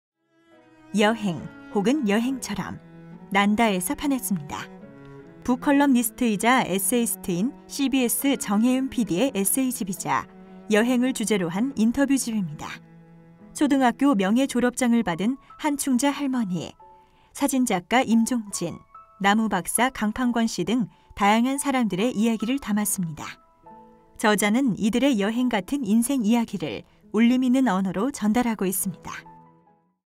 [온북TV] 여행, 혹은 여행처럼
CBS 정혜윤 PD 인터뷰 에세이집
초교 명예졸업장 받은 한충자 할머니, 나무 박사 강판권 씨 등
다양한 사람들의 이야기 울림 있는 언어로 전달해